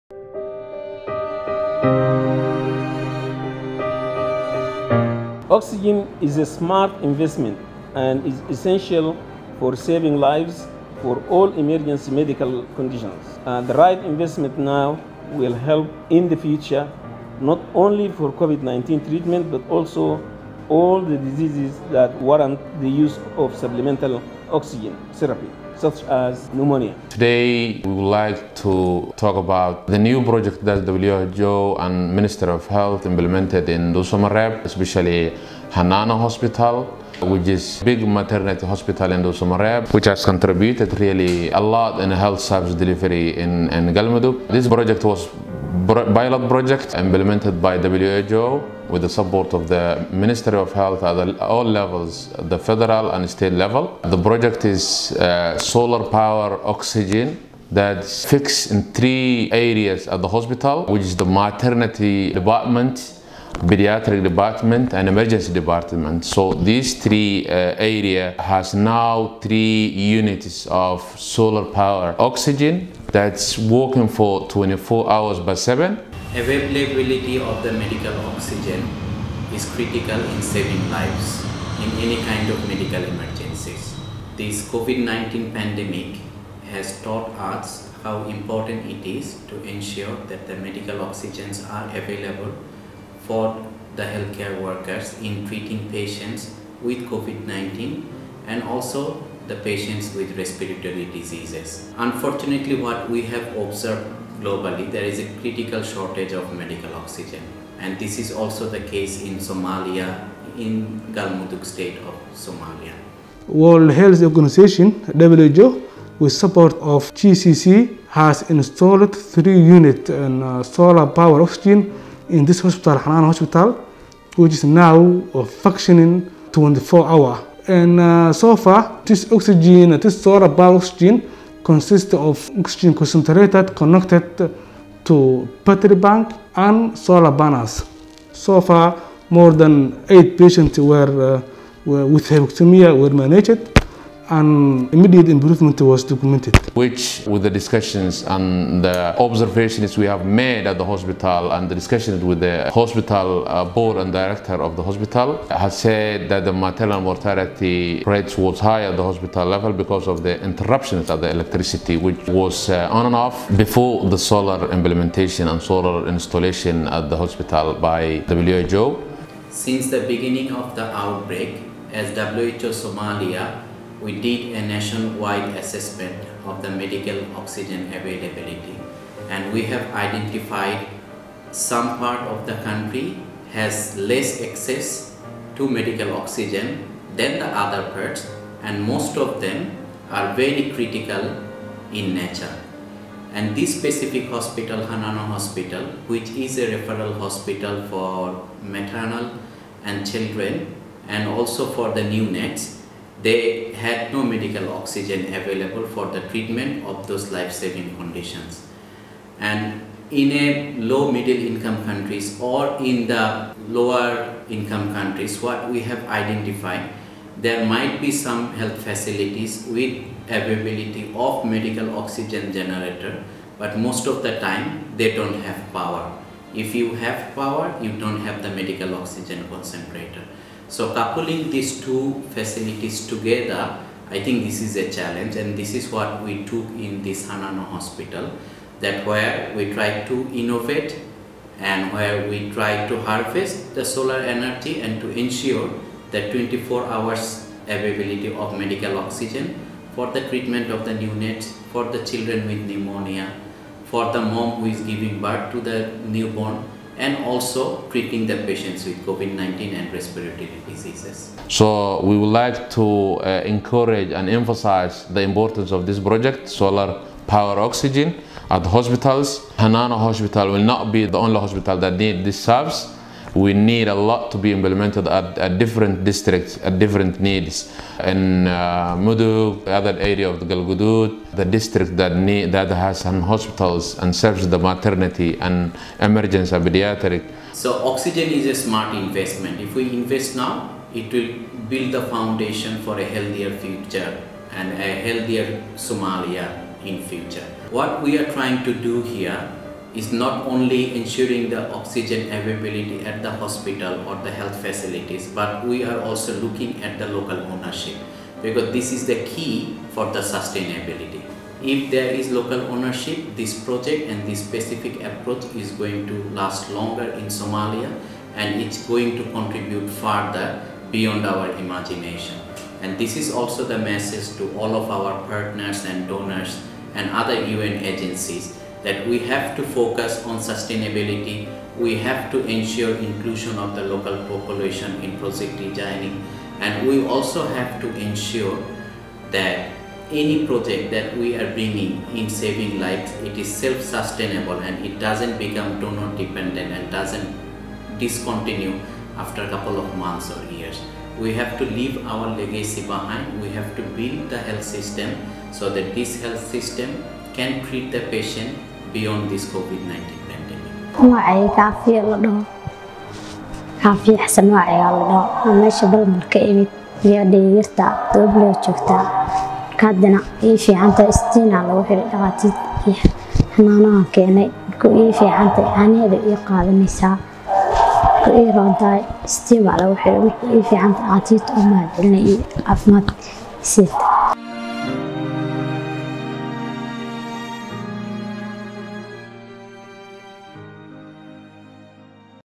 Somalia Solar-Powered Oxygen Concentrator Innovation
A significant milestone for WHO on its mission to sustainably scale innovations
in Somalia for improved health impact. Saving lives by scaling innovation in health.